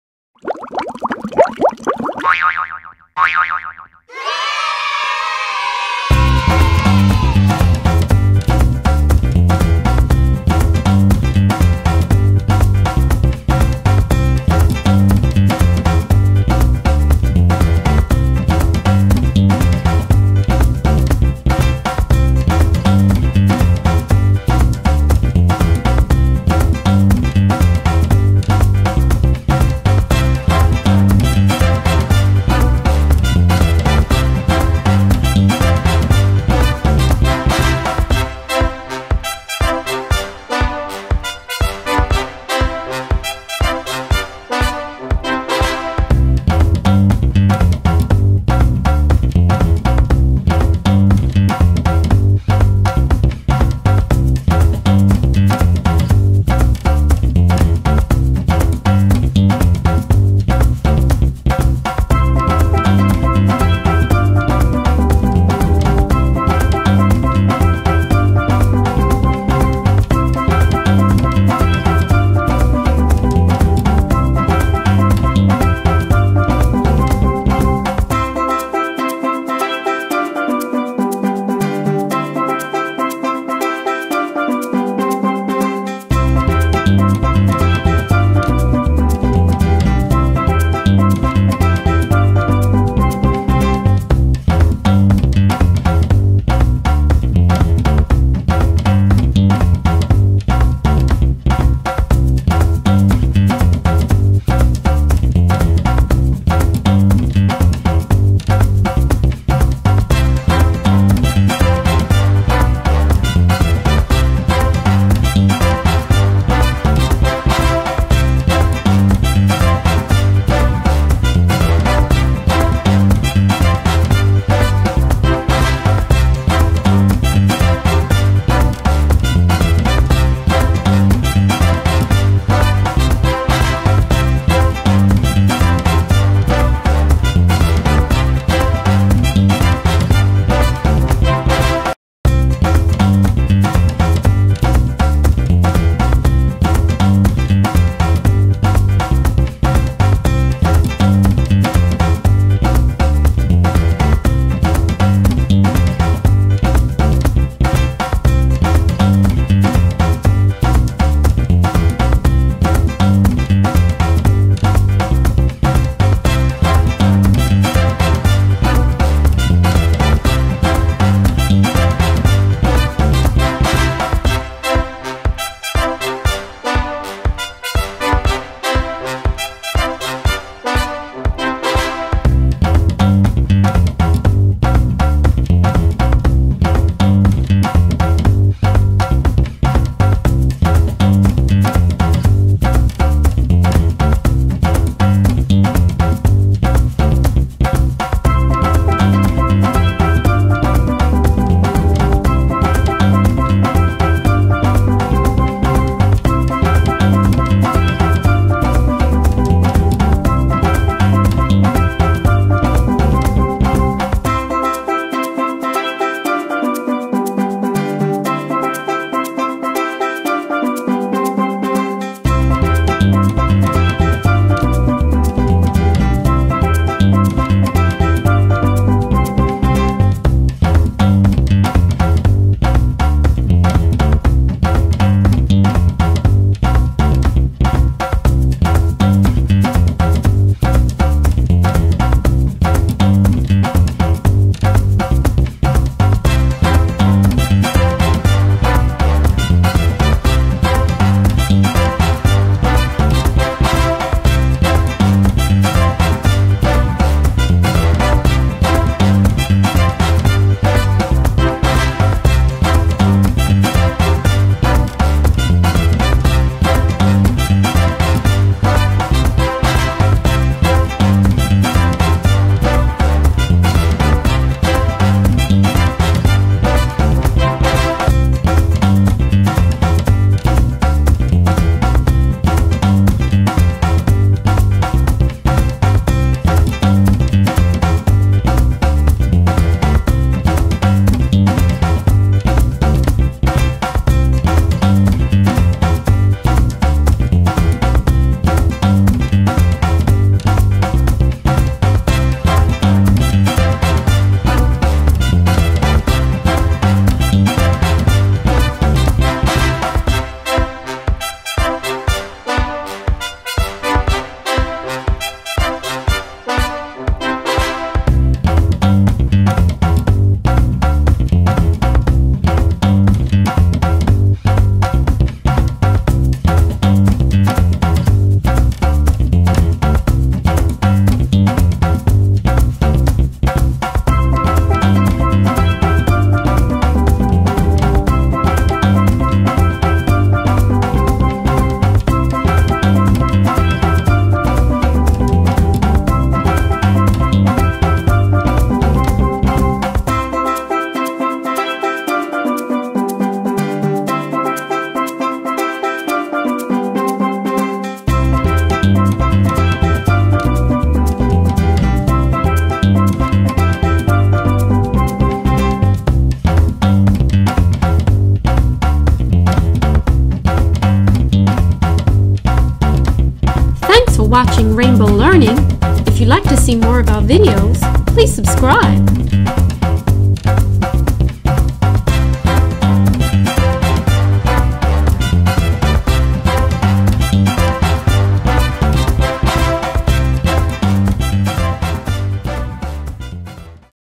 How to Make a Rainbow Heart Popsicle * Creative for Kids
Making a pink rainbow heart ice cream popsicle out of Play-Doh. Creative fun for kids.